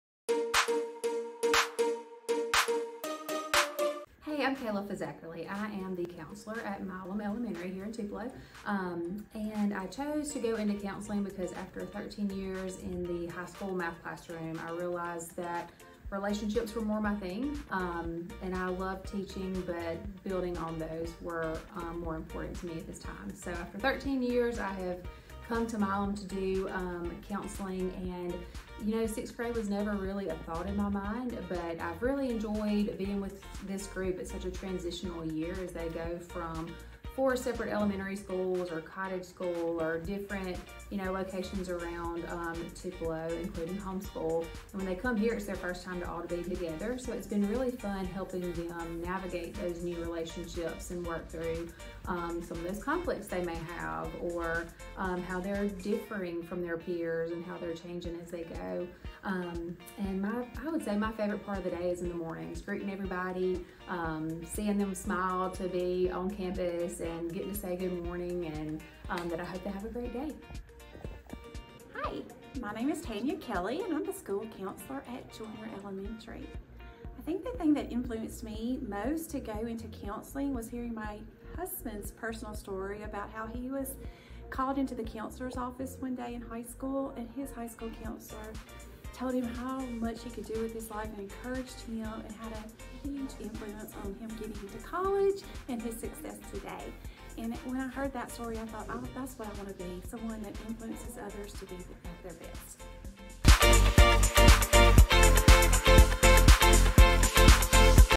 TPSD is celebrating National School Counseling Week!
Talking to a couple of our school counselors to see what inspires them to do what they do!